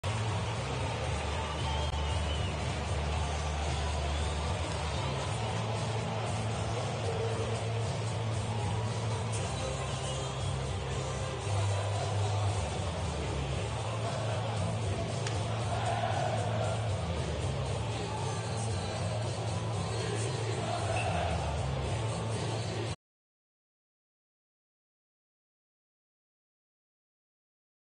Adam Edstrom pots one for Rogle
NYR prospect, Adam Edstrom  (2019 Rd6) with the snapper